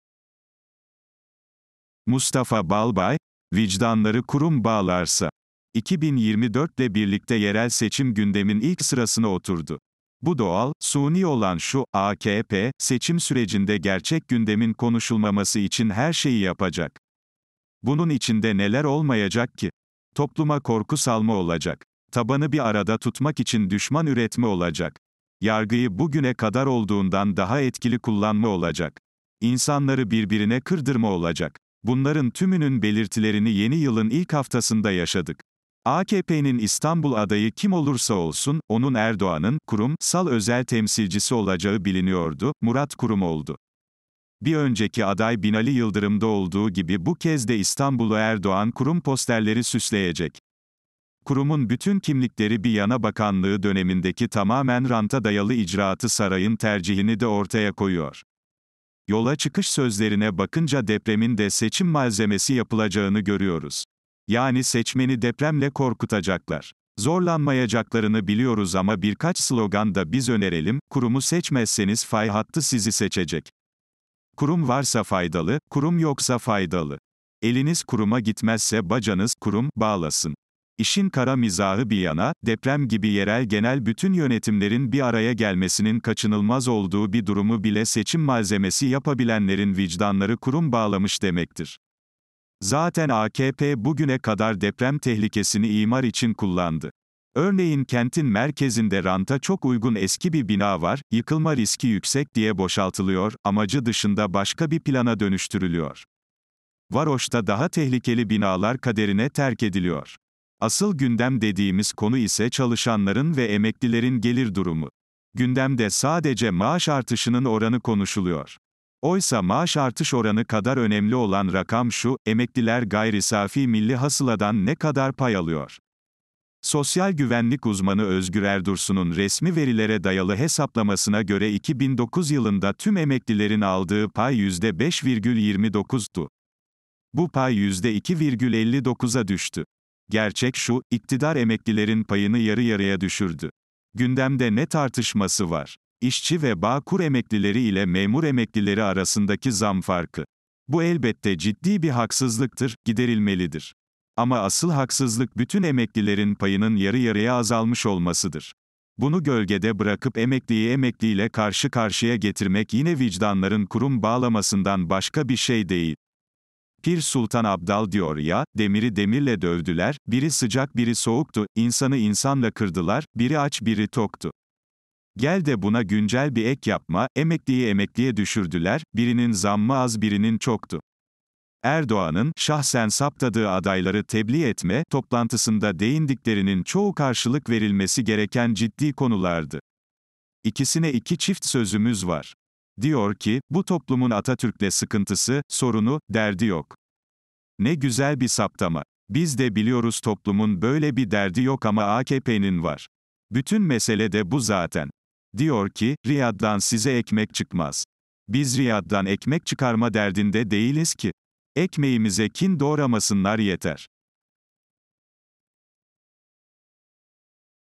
MUSTAFA BALBAY: VİCDANLARI KURUM BAĞLARSA - SESLİ KÖŞE YAZISI
Mustafa Balbay - cumhuriyet.com.tr - Vicdanları kurum bağlarsa - 9 Ocak 2024 tarihli yazısının seslendirmesi

"2024’le birlikte yerel seçim gündemin ilk sırasına oturdu. Bu doğal, suni olan şu:

AKP, seçim sürecinde gerçek gündemin konuşulmaması için her şeyi yapacak.

Bunun içinde neler olmayacak ki?

Topluma korku salma olacak...

Tabanı bir arada tutmak için düşman üretme olacak...

Yargıyı bugüne kadar olduğundan daha etkili kullanma olacak...

İnsanları birbirine kırdırma olacak...

Bunların tümünün belirtilerini yeni yılın ilk haftasında yaşadık...

AKP’nin İstanbul adayı kim olursa olsun, onu. "